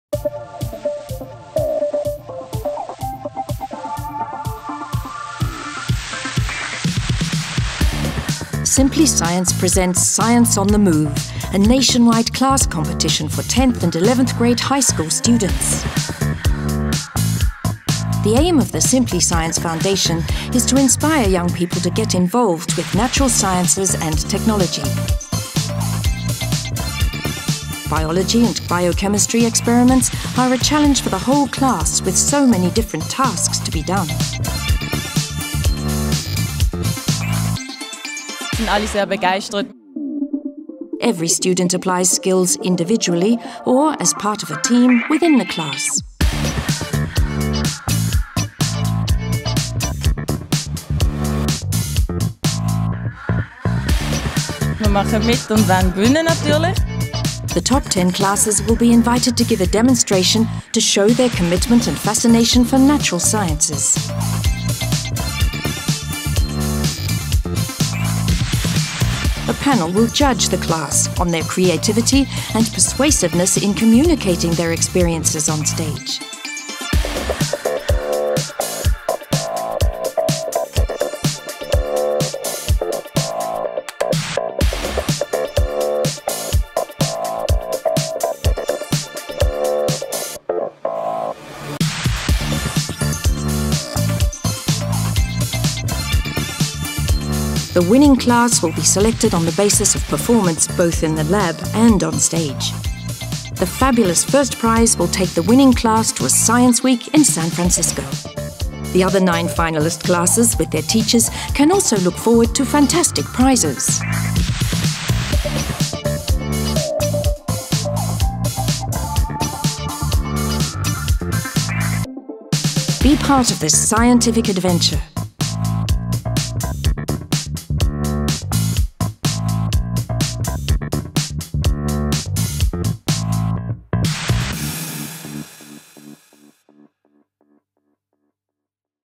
Science on the Move
A nation-wide class competition in biology and biochemistry with extraordinary top prizes to win...